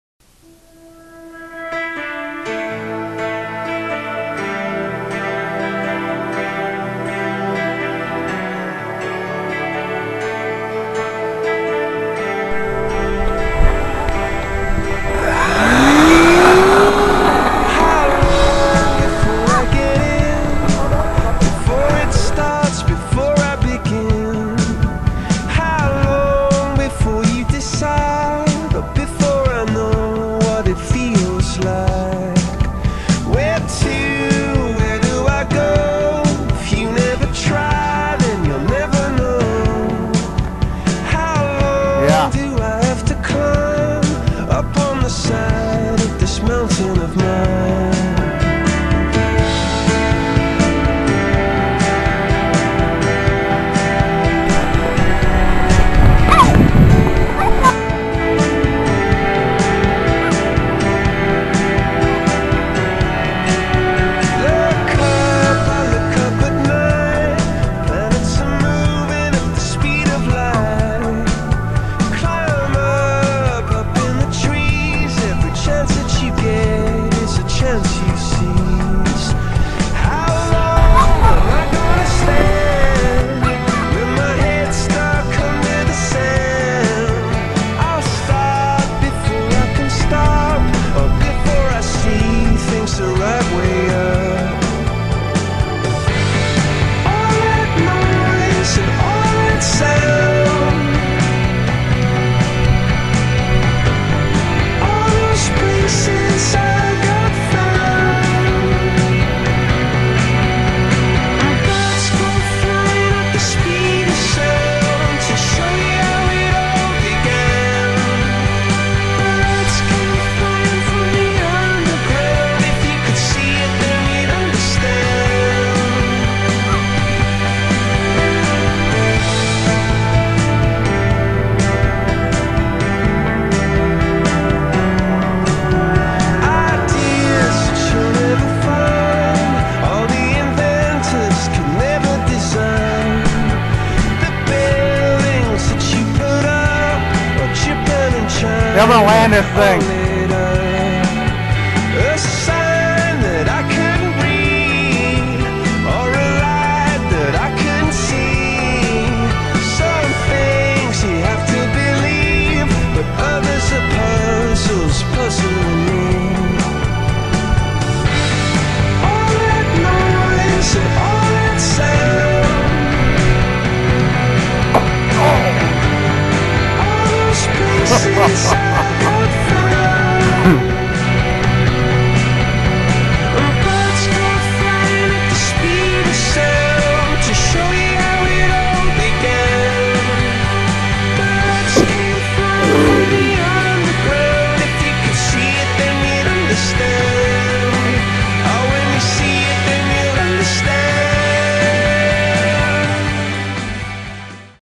3rd flight of my F 20 0001
Still testing out my F-20. I added a basic paint scheme for this flight but I am in the process of changing it to be more visible. I have also added K-F airfoils.